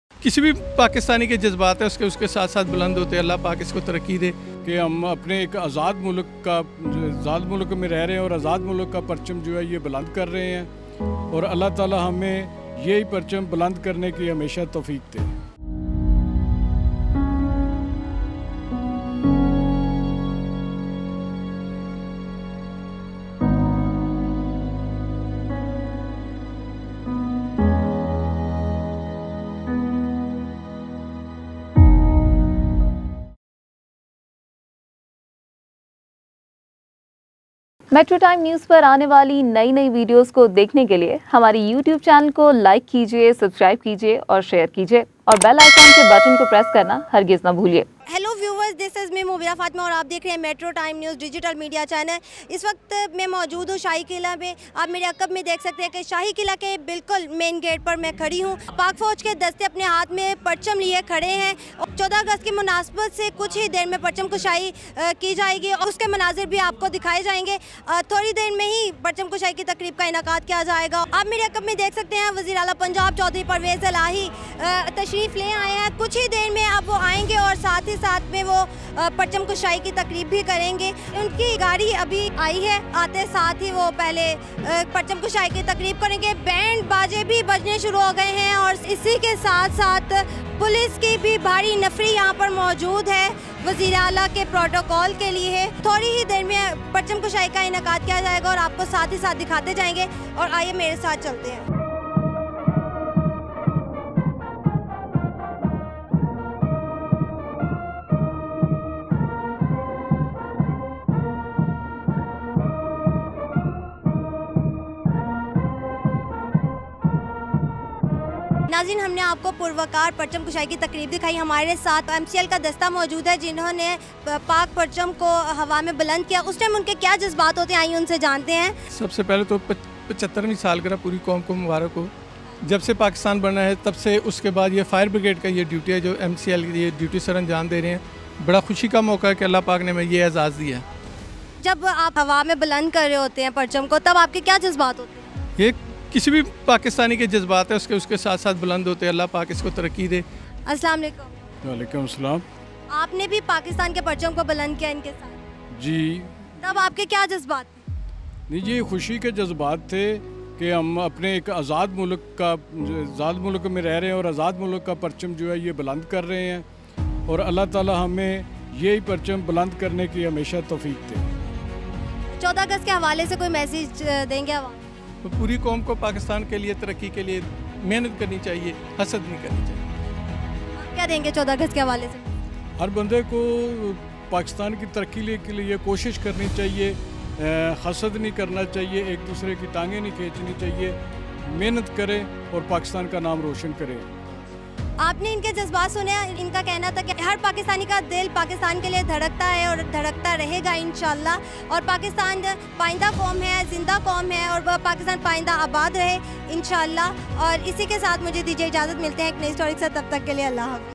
MCL ka dasta pakistan sy ly kr ab tk shahi kila main purcham kushai krny main masroof
| Hashtags |